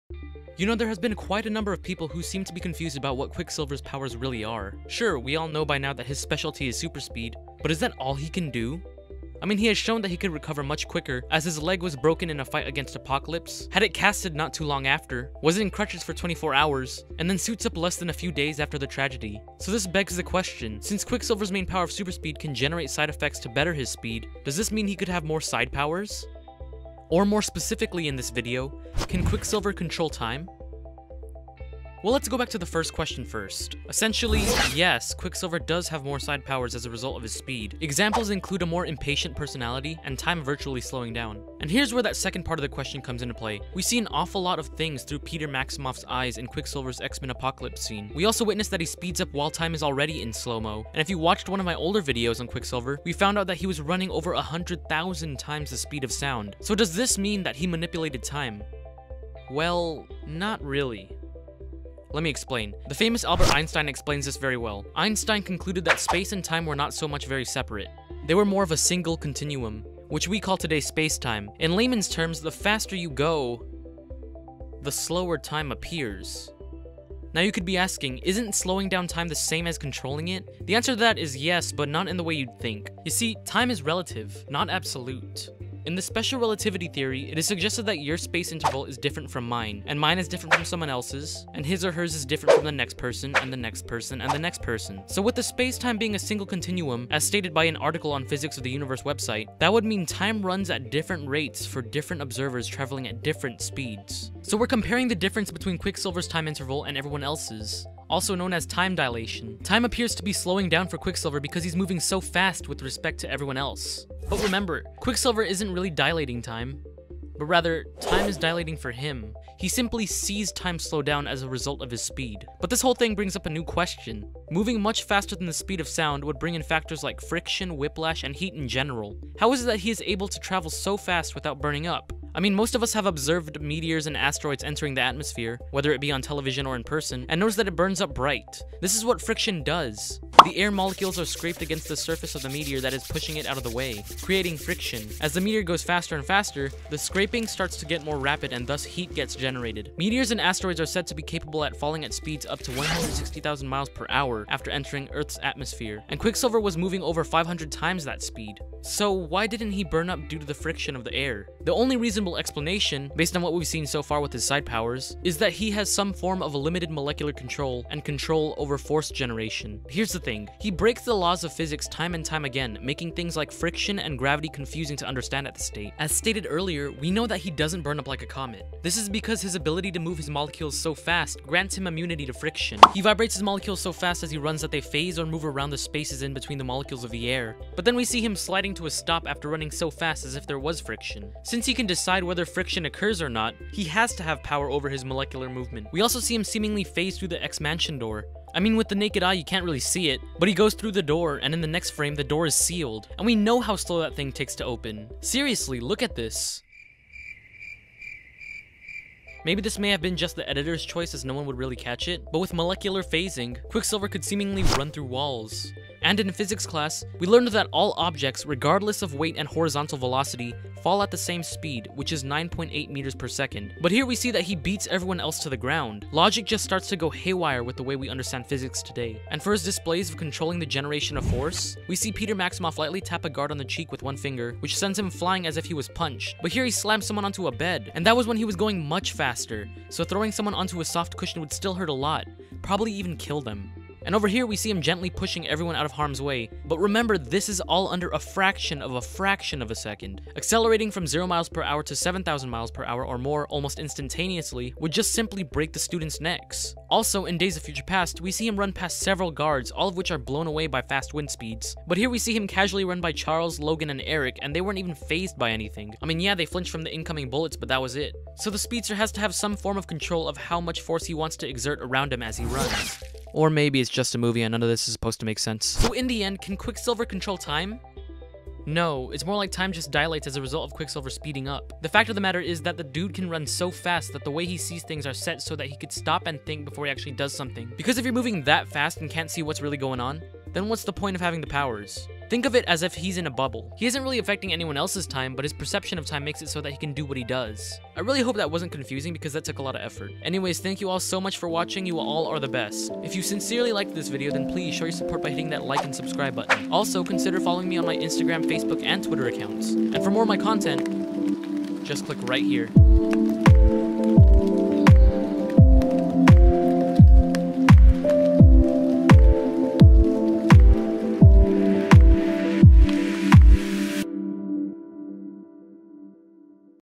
Can Quicksilver Control Time?
We all know what the mutant Peter Maximoff is capable of. He has shown that he can save over a dozen of mutants in a fraction of a second. But, does Quicksilver run really fast or slow down time? Find out now!

If you liked this video, then stick around!